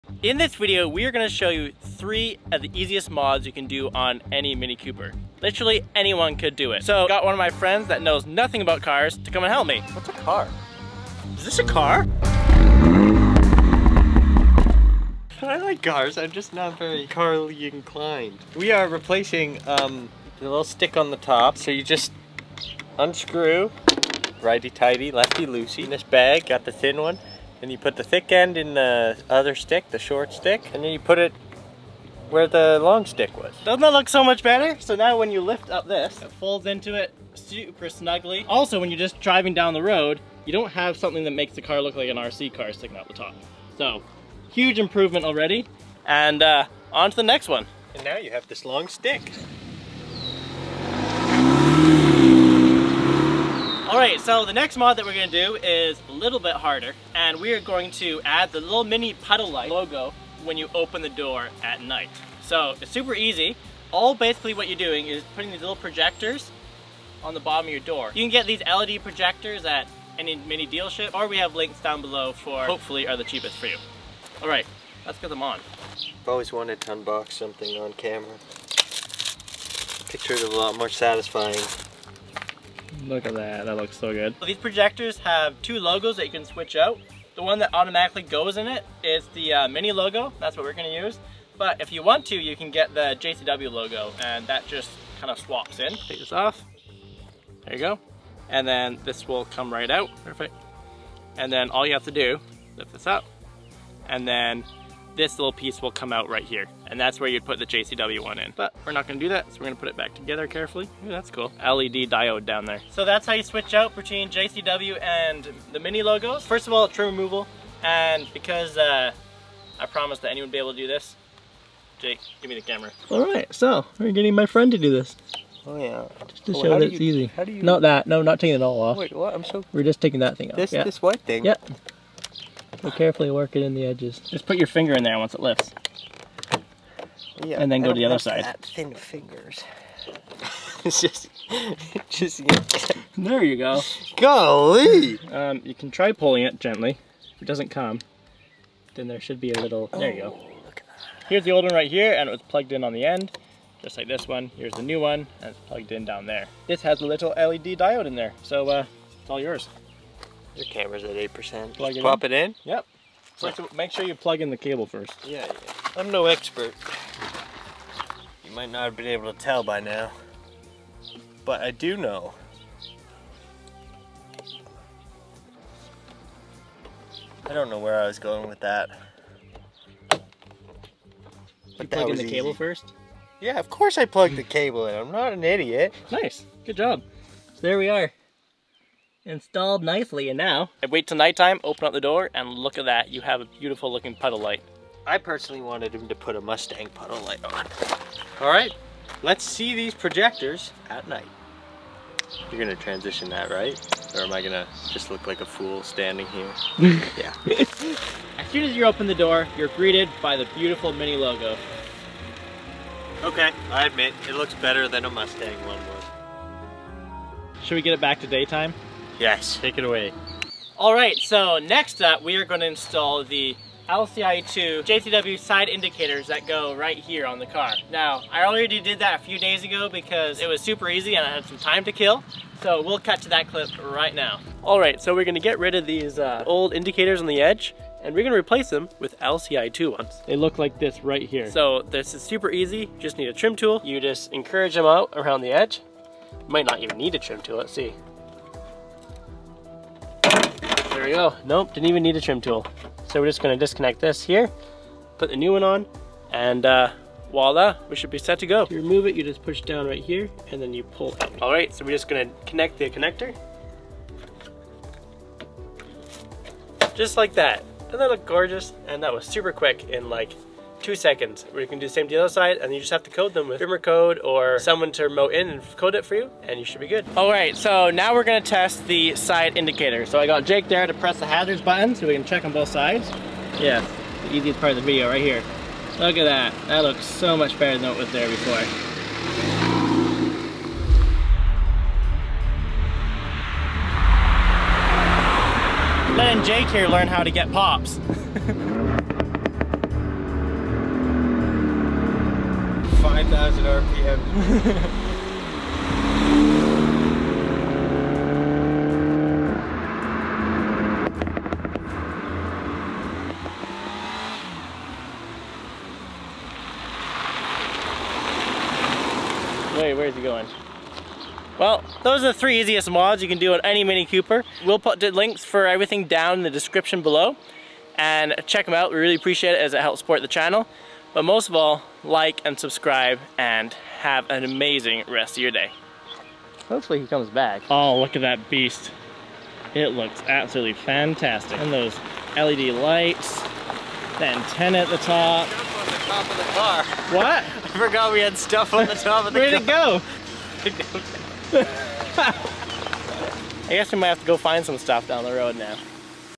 Try these three EASIEST MINI mods NOW!
Try three of the easiest mods you can do to ANY mini cooper!
If you want easy cheap mods for your car and worry if you can install, this is the video for you! We got my friend who knows nothing about cars to perform this DIY modification himself!